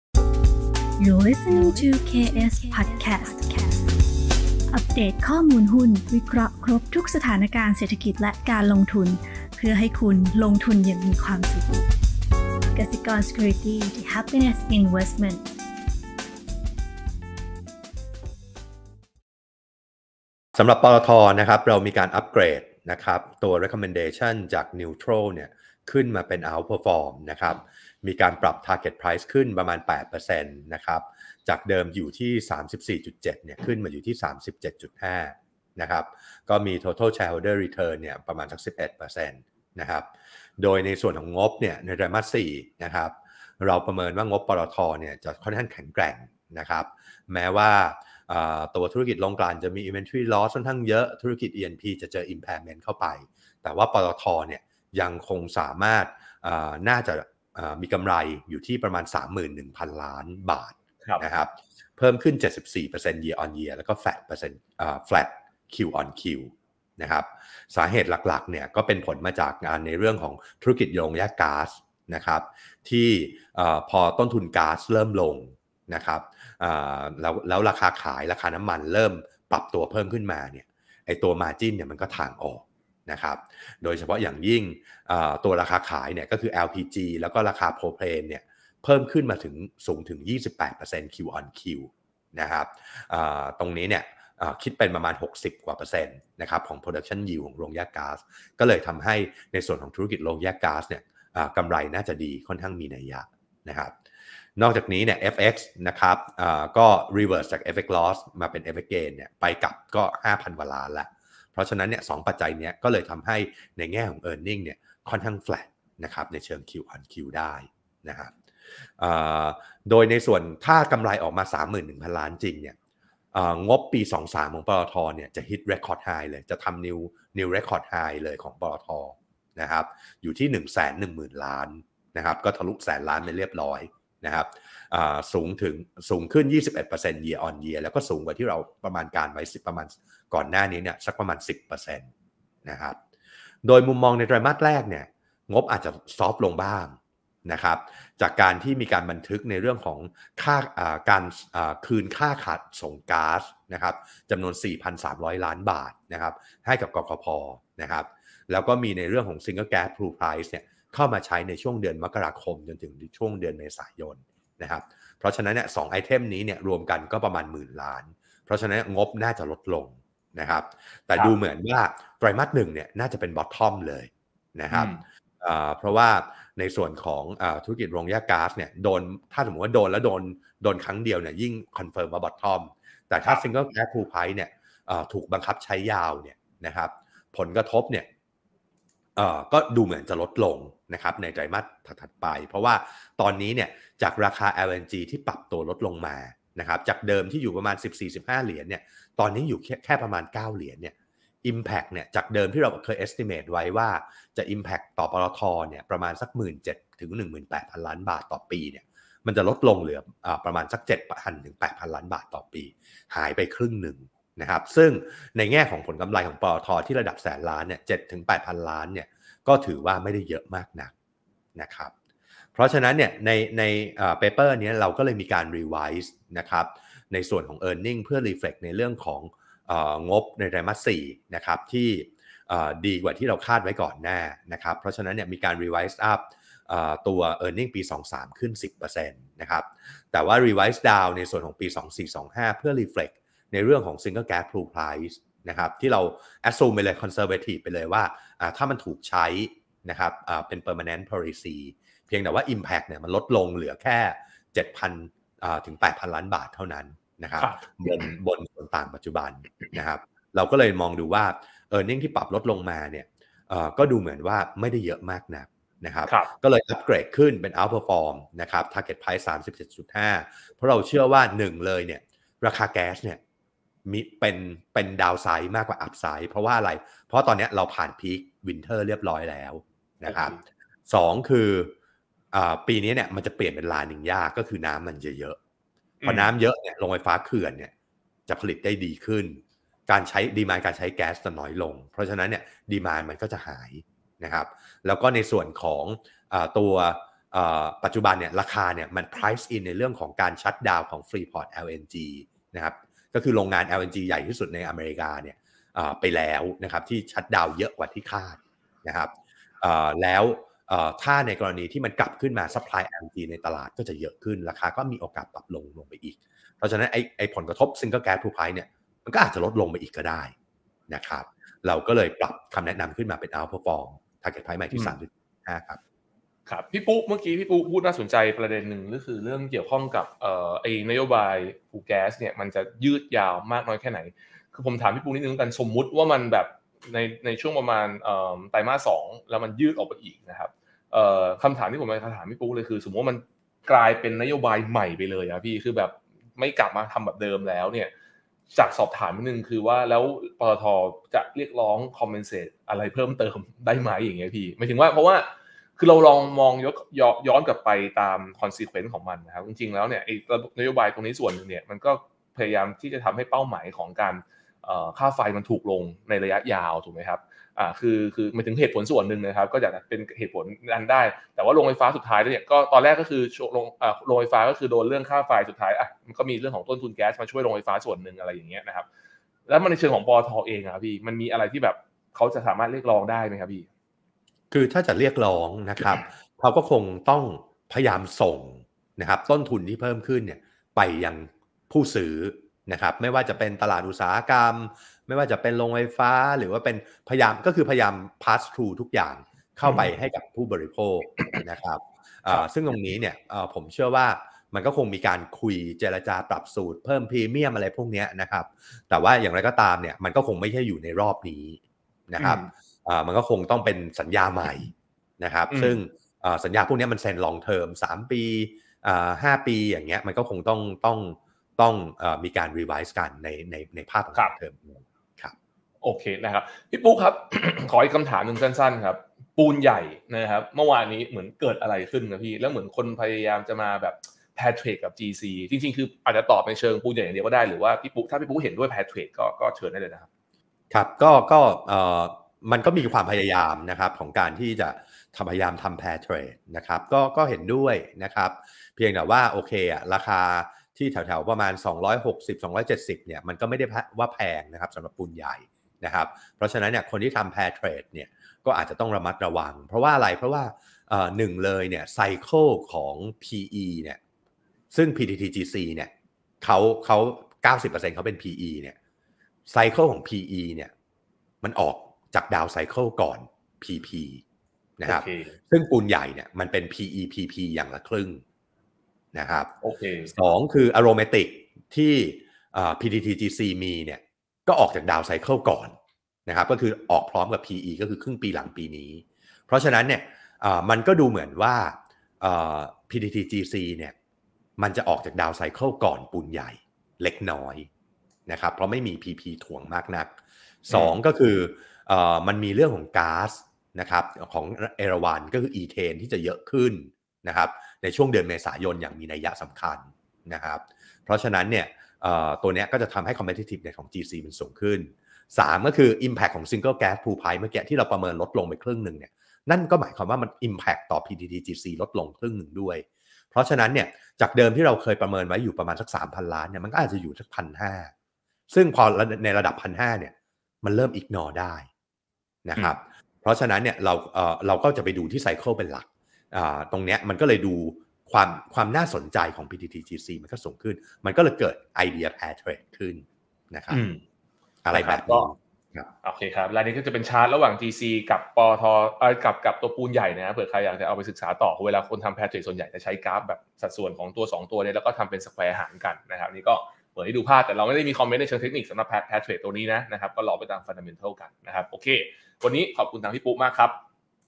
PTT คาดกำไร 4Q66 สูงสุดเป็นประวัติการณ์หลังผลดำเนินงานโรงแยกก๊าซดีขึ้น
(8 ก.พ. 67)